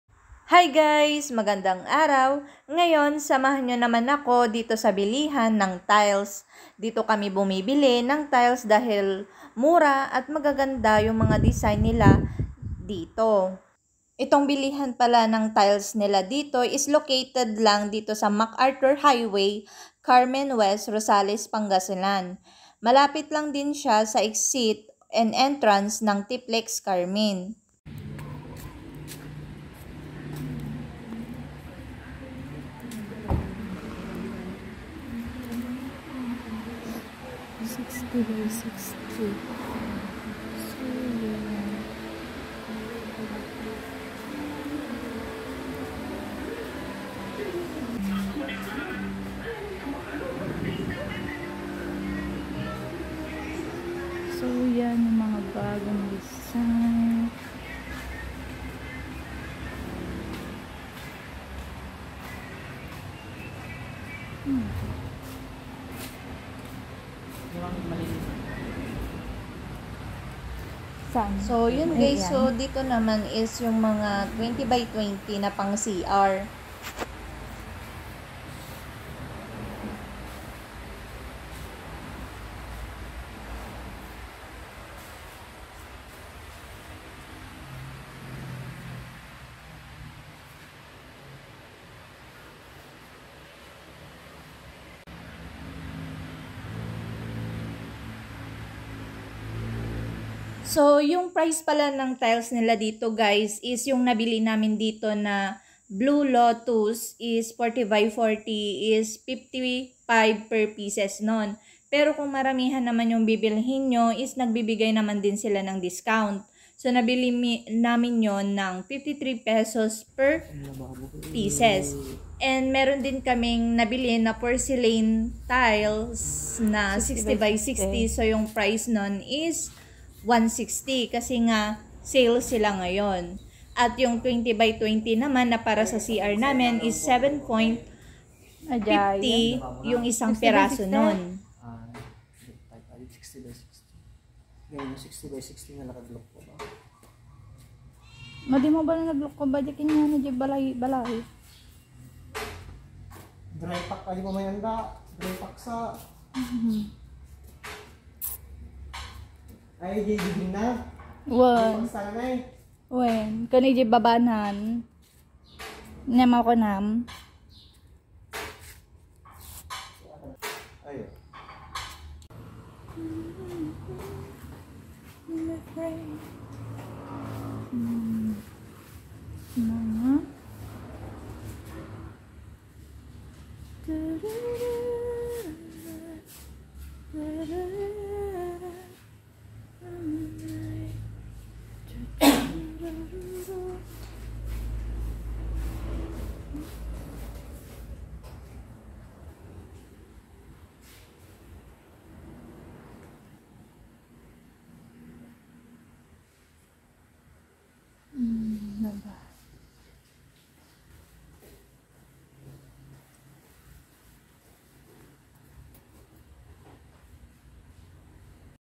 Warehouse Sale! Murang Bilihan ng Tiles Carmen West Rosales Pangasinan
Murang Bilihan ng tiles dito lang yan sa MacArthur Highway Carmen West Rosales Pangasinan

60x60= 160
40x40= 55
20x20= 7.50